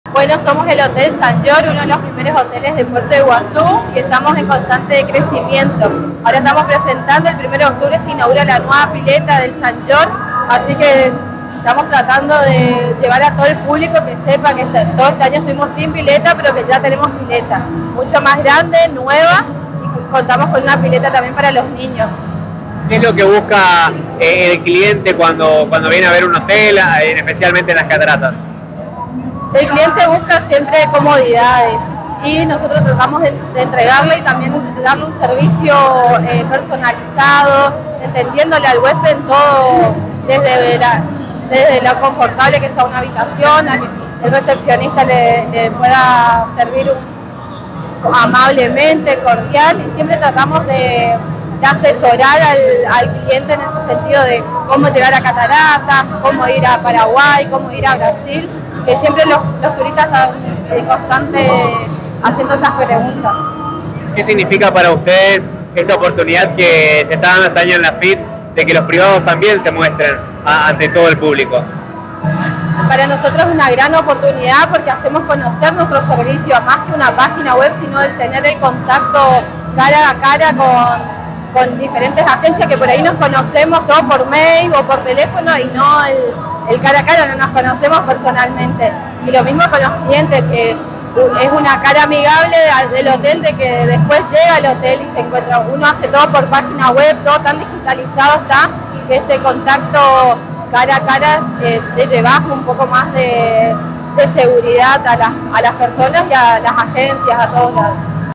FIT 2024 El hotel Saint George renueva su oferta y la presentó en la feria
El Hotel se presentó en la FIT 2024 y dio a conocer las inversiones y mejoras que preparó para esta temporada.